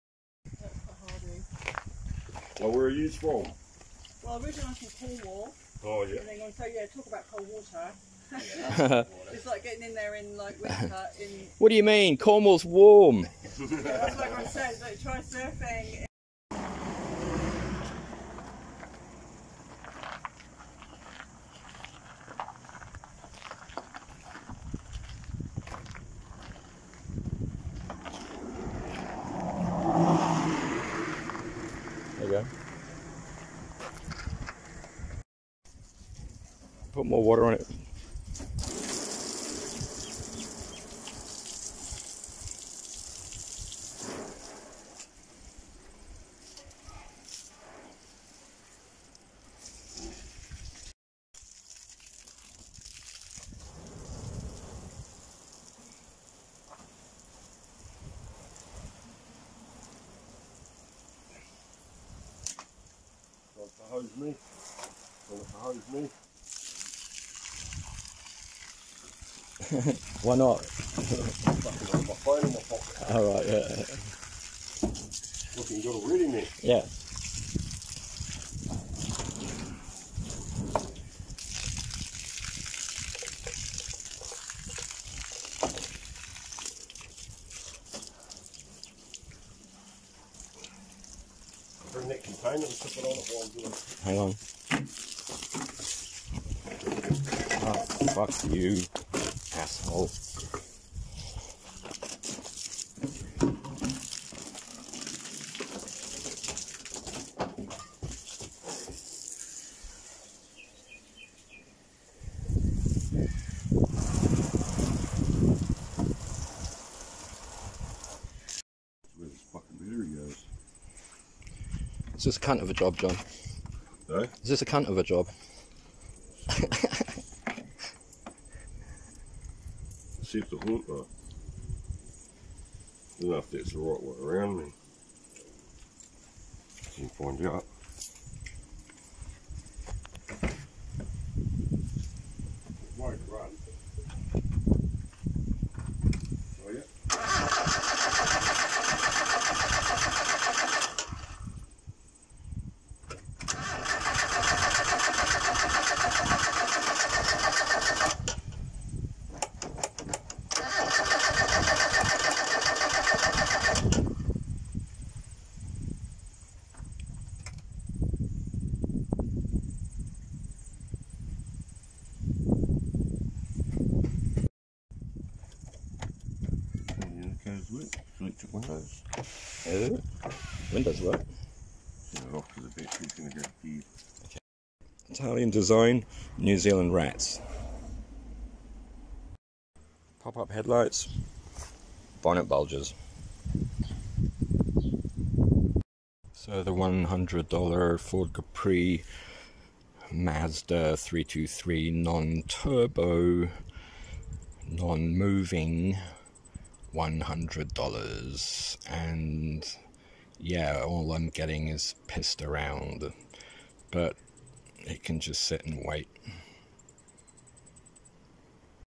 $100 1991 Ford Capri Mazda 323-Shed find-first start and profit??? Pt.1
Bought this parody of great Aussie engineering..sold in the US and Asia..domestic market and then a few made it to NZ.Towed it home .1600 DOHC -FWD-non turbo,Lots of Italian design. Fuel pump is stuffed so wont run.Sorry for shit production-Filmed on the phone.Whanganui NZ.
Now trying to sell the bastard.More videos of scrappie classic cars and auctions coming. Im a really bad self promoter.
Thanks.Rab